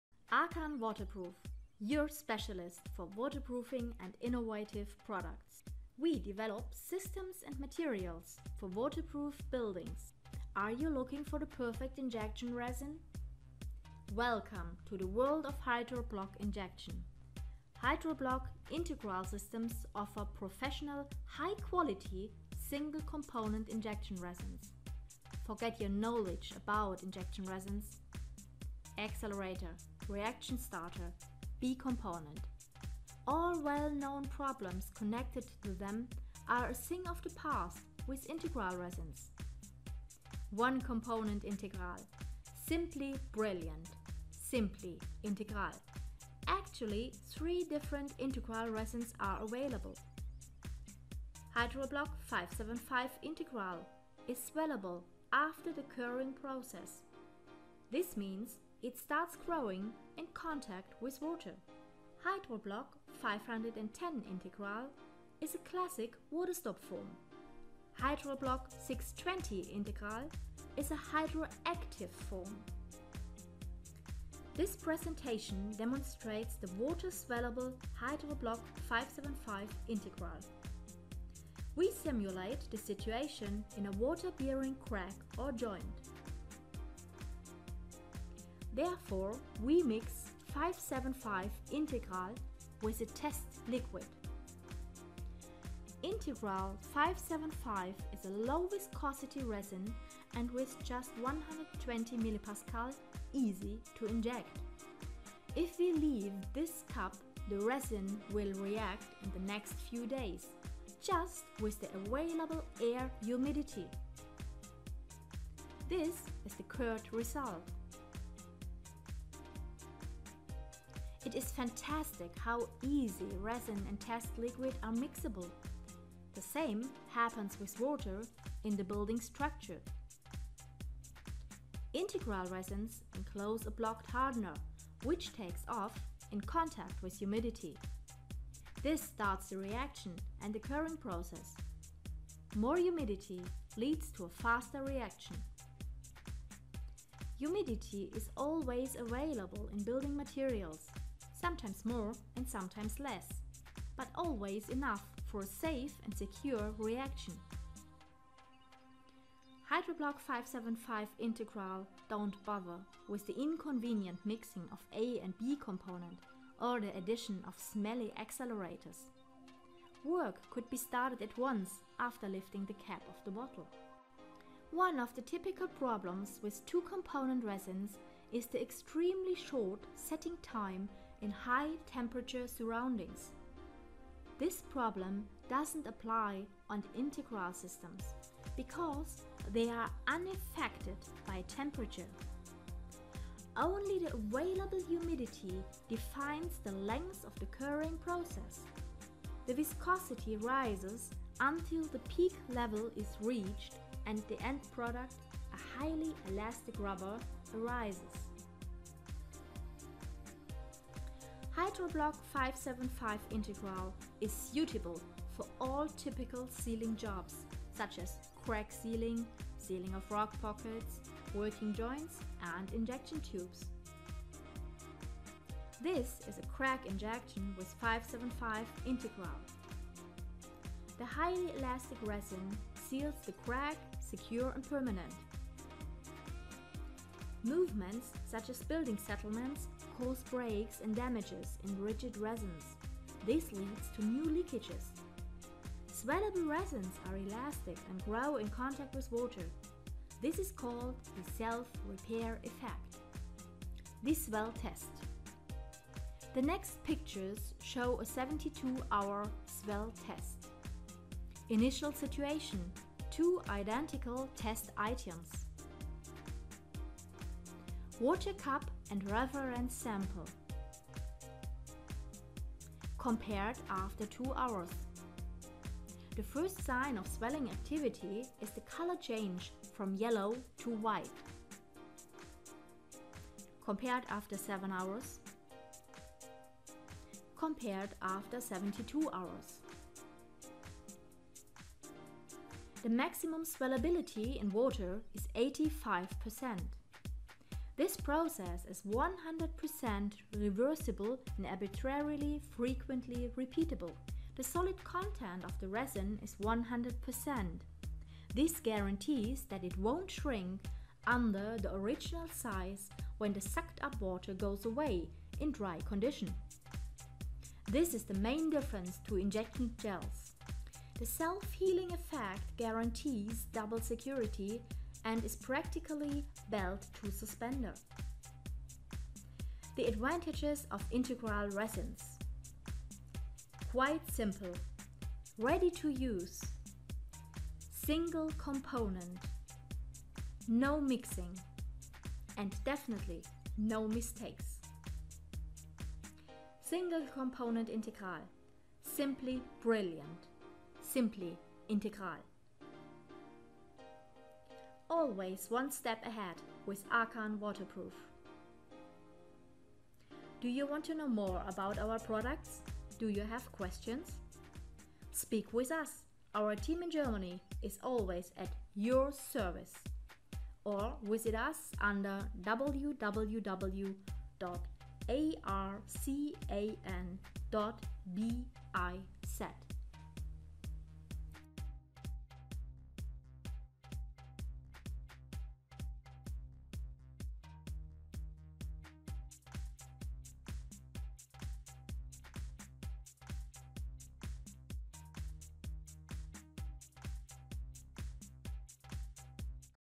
HydroBloc 575 Integral - the swellable Integral system
HydroBloc 575 Integral - 1 component PUR, ready to use injection resin!
Integral resins are ready to use without mixing. They don´t need any accelerator or reaction starter.
Integral resins make injection easy, efficient and save.
HydroBloc 575 Integral is a water swellable system, it grows after the curing process in contact with humidity. It is not a waterstop foam!